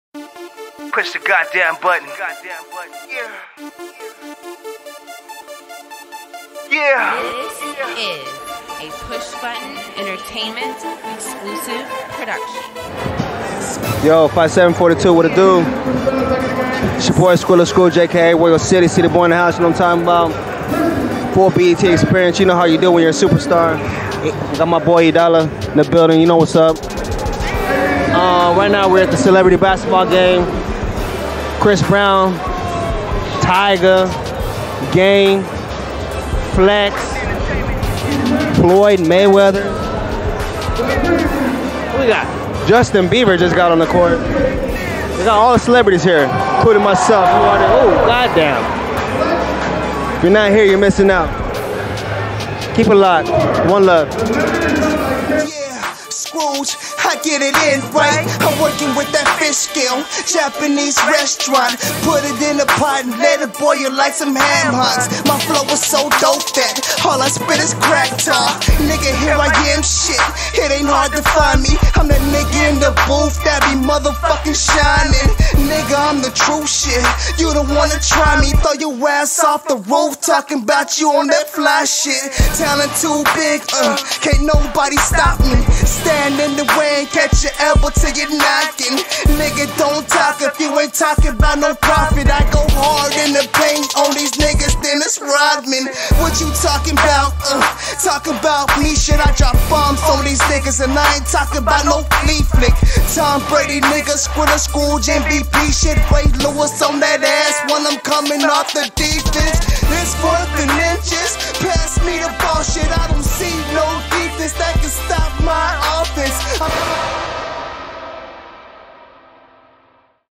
Skrilla Scrooge live at the B.E.T Celebrity Basketball Game
All New ReMastered Sound !!! by International  super Star Skrilla Scrooge mastered by universal music. push button ent. skrilla scrooge and edreick stitts